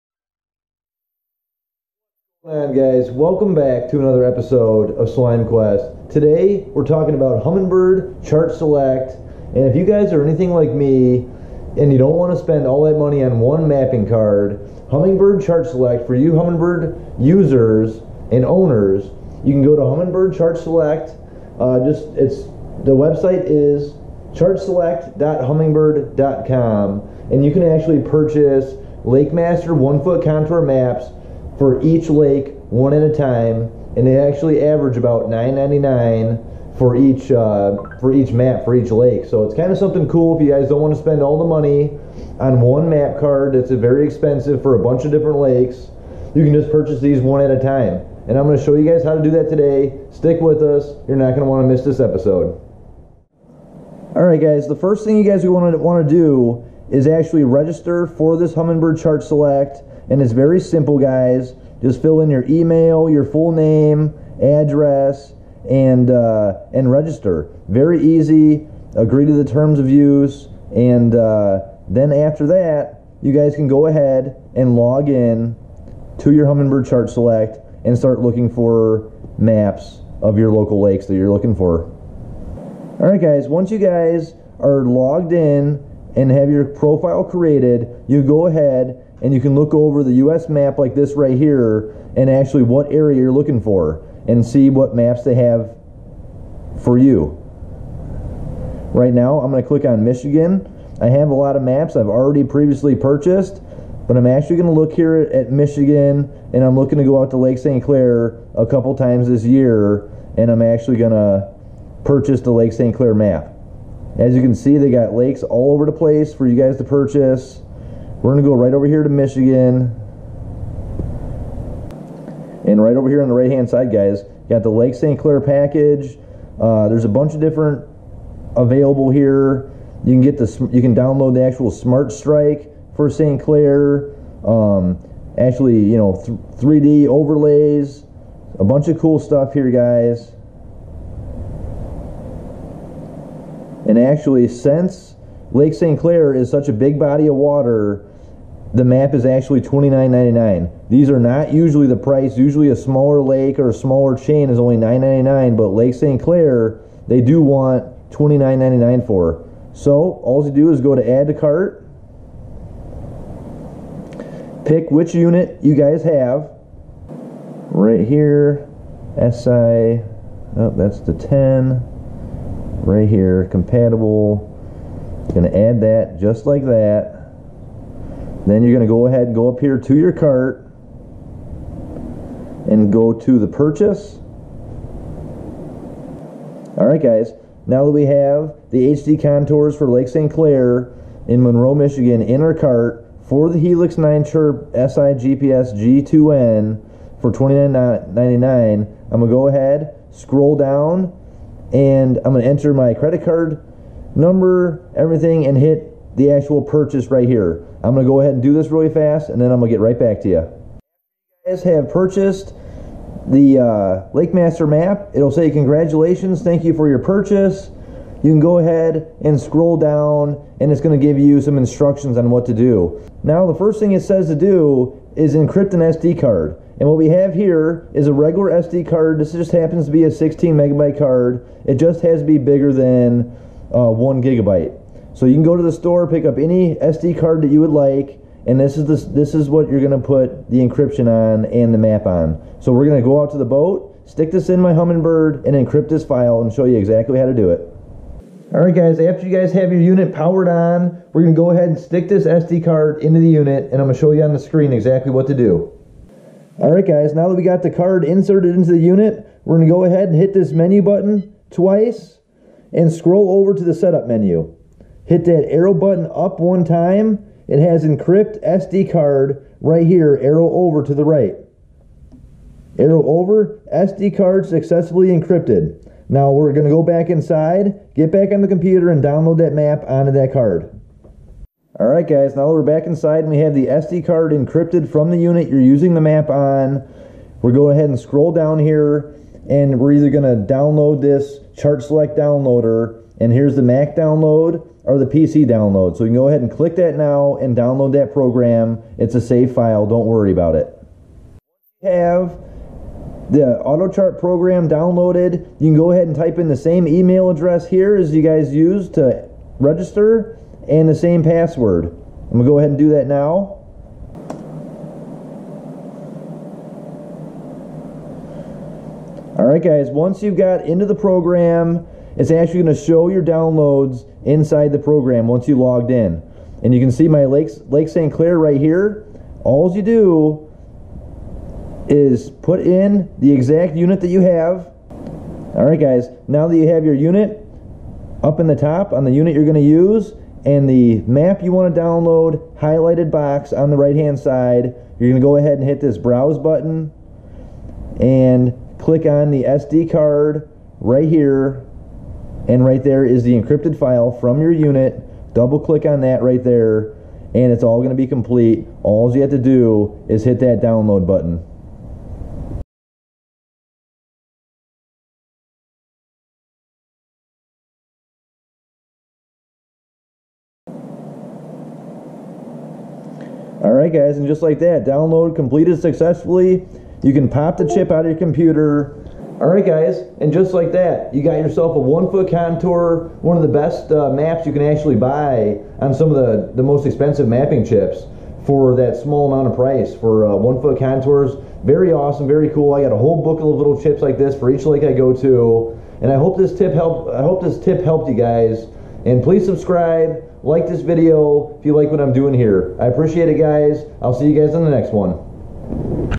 Humminbird Chart Select - Purchase your individual HD Lake Maps
Humminbird has been making some of the best fishing sonars in the industry! Humminbird Chart select is taking it to the next level by letting you download 1 foot contour HD maps of individual lakes one at a time for a low price.